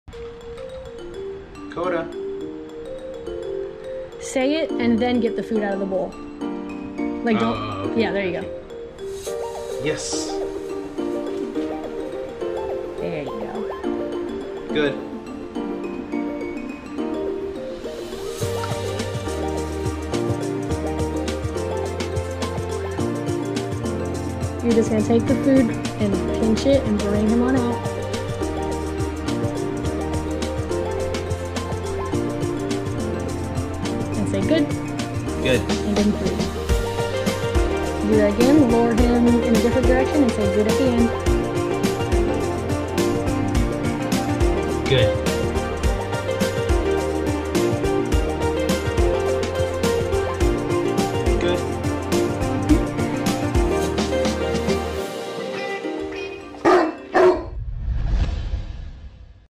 3 WORDS TO SAY WHEN HAND FEEDING A PUPPY
Koda is 3 month old Australian Cattle Dog that is learning 3 important words while his owner feeds him. We recommend hand feeding puppies instead of always eating from a bowl so the owner can have the opportunity to use food as a reward system which will help develop a puppy's "working state of mind".

Yes - a release word to let your puppy know they have permission to be free

Good - a word to give positive feedback when your puppy is holding a command

Your Dog's Name - Name recognition